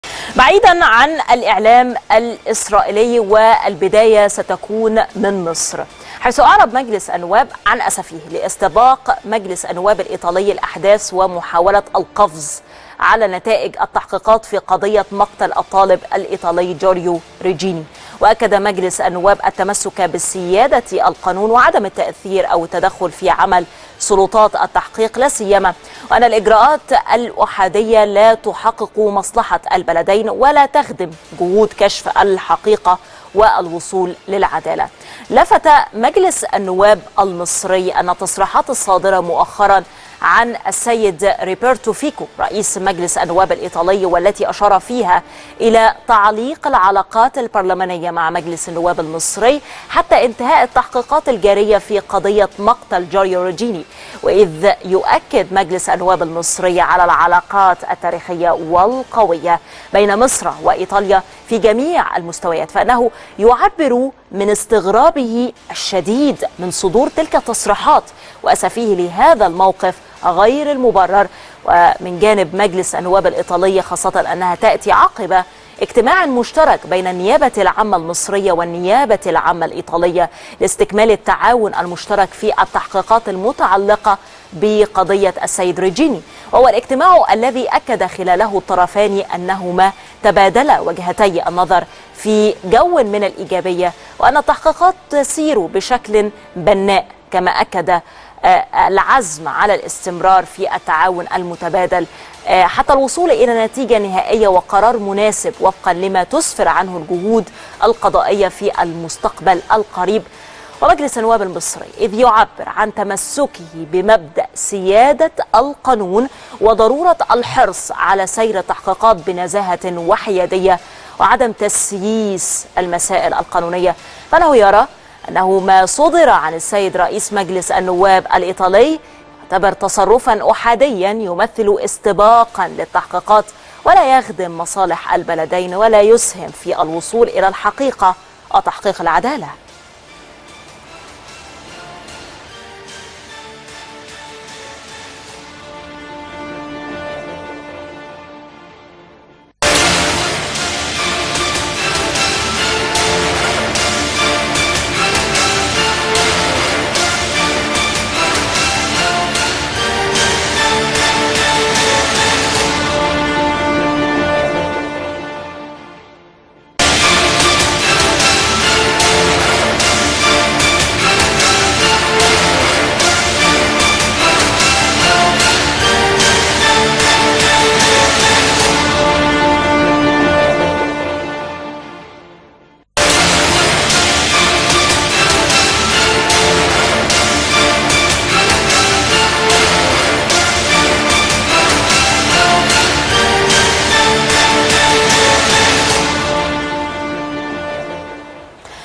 نقاط ساخنة| مجلس النواب المصري يؤكد على تمسكه بسيادة القانون في قضية ريجيني
نقاط ساخنة| الحكومة العراقية الجديدة بين التجاذبات الأمريكية | 29 نوفمبر 2018